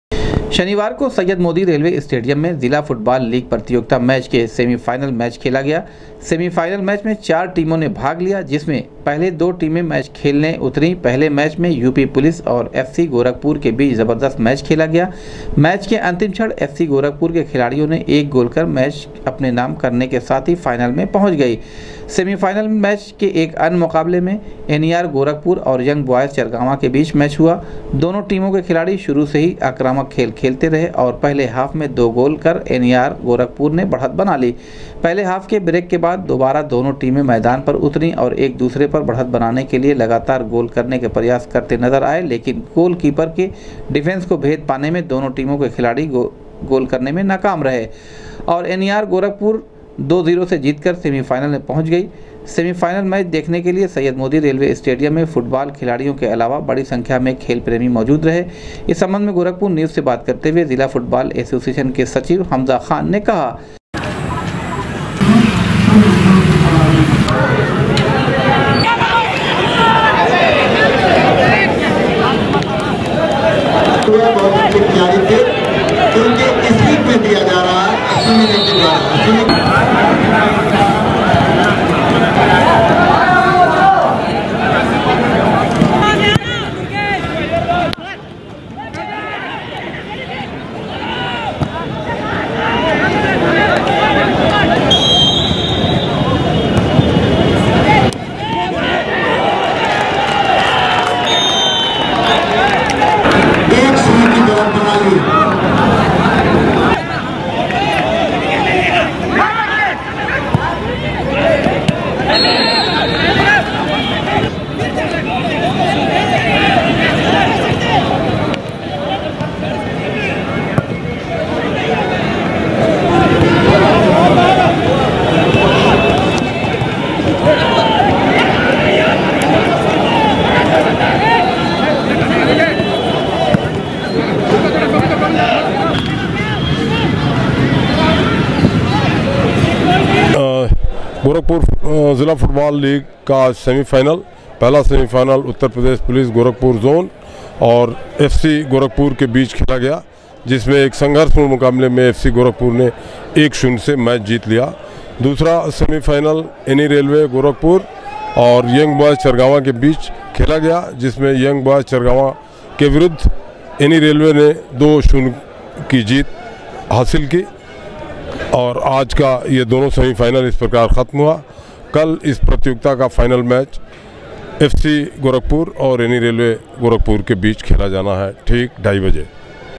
एफसी गोरखपुर ने यूपी पुलिस को और एनईआर ने यंग ब्याज चरगावां को हराकर पहुॅची फाइनल में
जिला फुटबॉल लीग में एफसी गोरखपुर ने यूपी पुलिस को और एनईआर ने यंग ब्याज चरगावां को हराकर पहुॅची फाइनल में, 22 सितंबर को खेला जाएगा फाइनल मैच GORAKHPUR NEWS  -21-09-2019